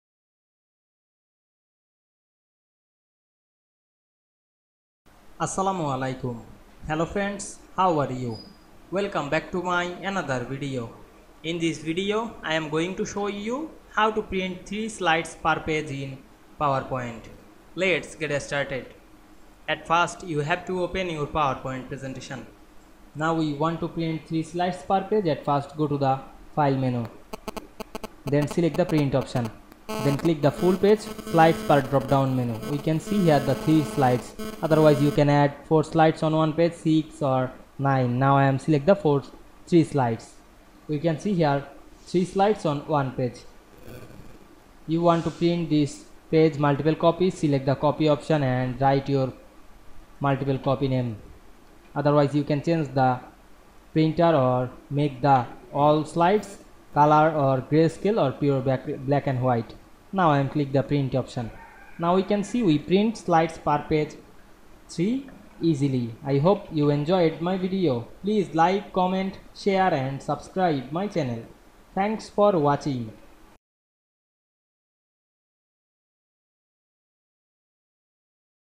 How to print 3 slides per page in PowerPoint
Assalamu Walaikum,
In this video I will show you, How to print 3 slides per page in PowerPoint.  Let's get started.